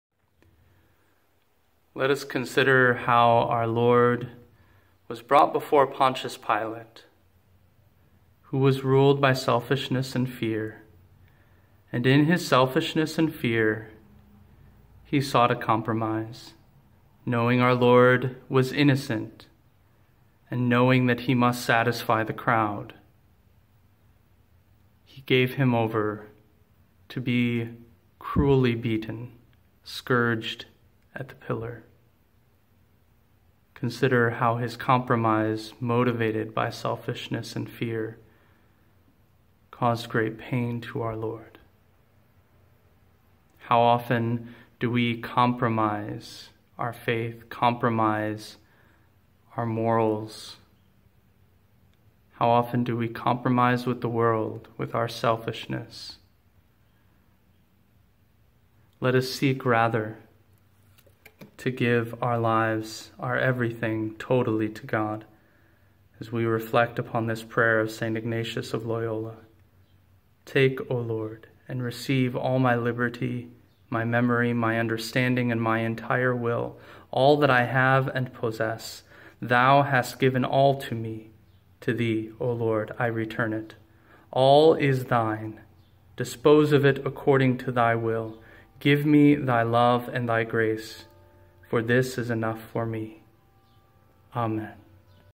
Compromises (2nd Sorrowful Mystery)
A "Minute-Prayer" reflecting on Second Sorrowful Mystery: The Scourging at the Pillar.

Using a prayer of St. Ignatius of Loyola.